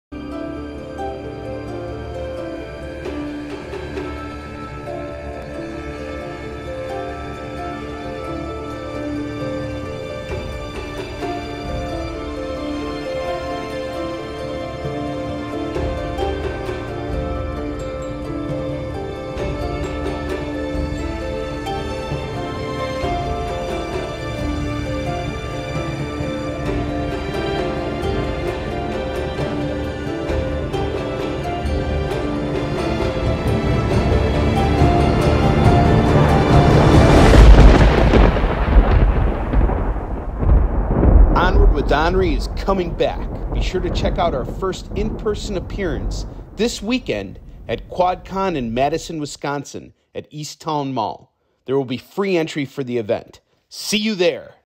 YI SOON SHIN RETURNS TO COMIC-CON AT QUADCON 2021 IN MADISON, WISCONSIN! FREE ADMISSION!
For those of you who missed this afternoon's episode of KICKIN' IT WITH ONRIE AND KEVIN: THE LIVE SHOPPING SHOW, we just announced that we are returning to the frontlines of comic-con THIS weekend (April 10th-11th) at Quad Con Comic & Toy Show / Cool Stuff in Madison, Wisconsin (FREE admission)!

Where we last left off before COVID hit, Yi Soon Shin was on track to 100 convention sell outs. We're just 12 shows away from reaching this very important milestone for our series! We're getting ready to sling 700 books in two days. Is it possible? Find out as we bring back ONWARD WITH ONRIE next week!

Hunter Killer Collectibles will be setting up shop too. We'll have 10 short boxes full of random stuff going for pennies on the dollar!

Looking forward to seeing you guys there!

ONWARD TO VICTORY!